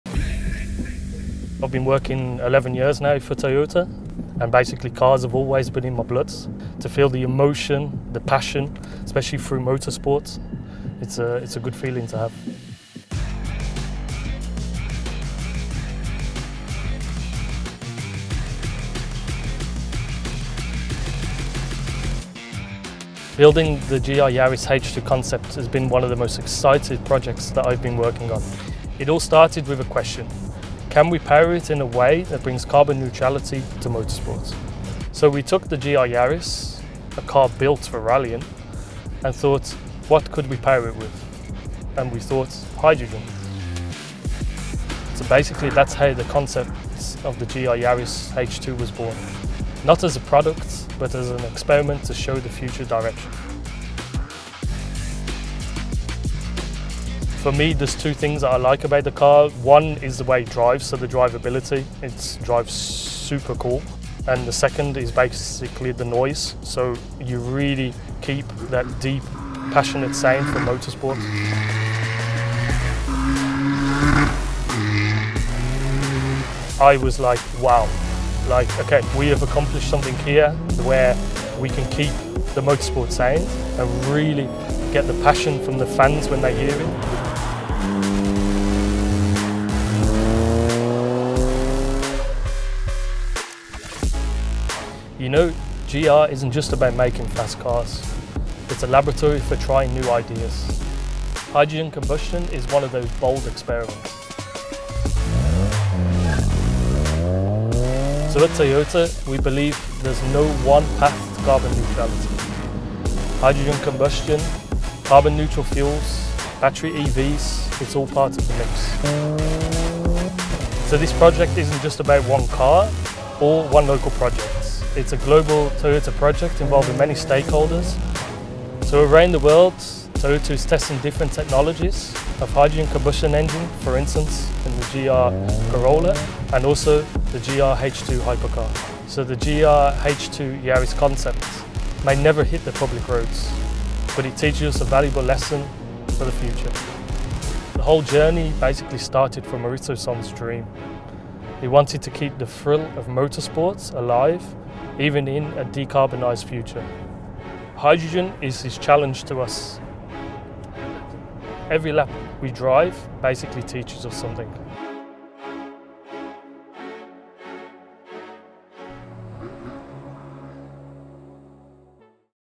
Uhlíkově neutrální motorsport?! Představujeme GR Yaris na vodík
Vzali jsme Toyotu GR Yaris, auto postavené pro rallye, a zkusili mu dát vodíkový pohon. Tak vznikl koncept GR Yaris H2. Experiment, který ale jasně ukázal, že i závodní auta mohou jezdit zcela bez emisí.

A víte, co je nejlepší? Hluboký zvuk, který k motorsportu neodmyslitelně patří, zůstává, stejně jako skvělá ovladatelnost a požitek z řízení.

Nahlédněte do zákulisí vývoje tohoto výjimečného vozu, který je zároveň technologickou laboratoří budoucnosti.